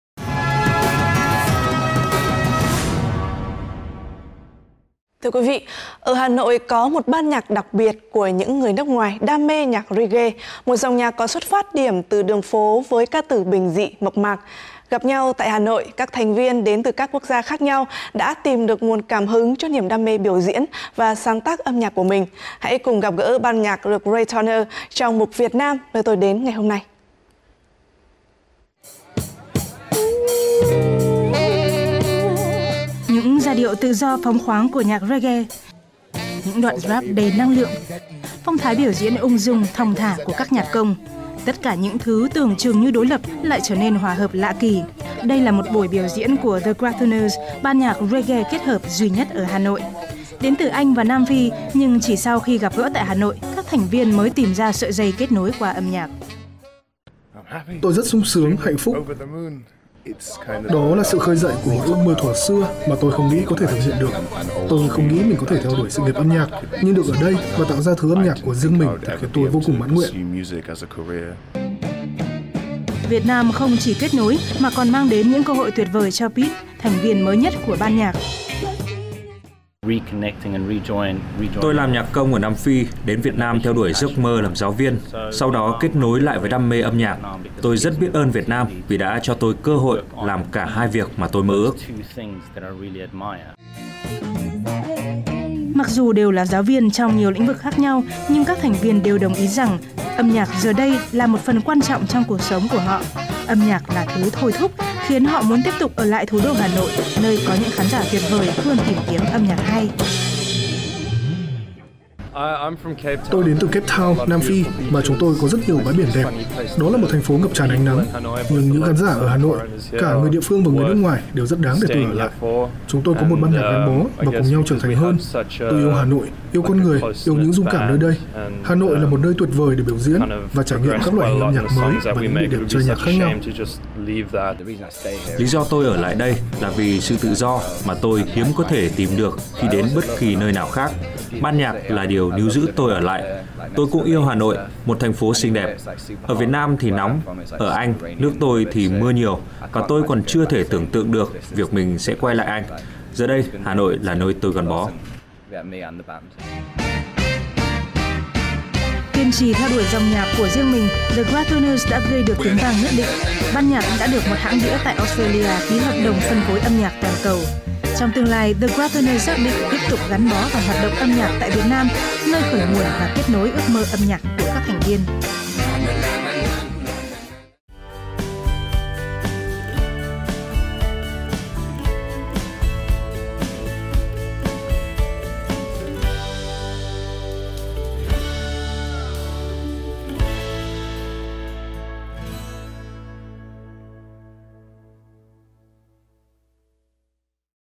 Hà Nội kết nối những nhạc công Reggae | VTV4
Ở Hà Nội, có một ban nhạc đặc biệt của những người nước ngoài đam mê nhạc reggae, một dòng nhạc có xuất phát điểm từ đường phố, với ca từ bình dị, mộc mạc. Gặp nhau tại Hà Nội, các thành viên đến từ các quốc gia khác nhau đã tìm được nguồn cảm hứng cho niềm đam mê biểu diễn và sáng tác âm nhạc của mình.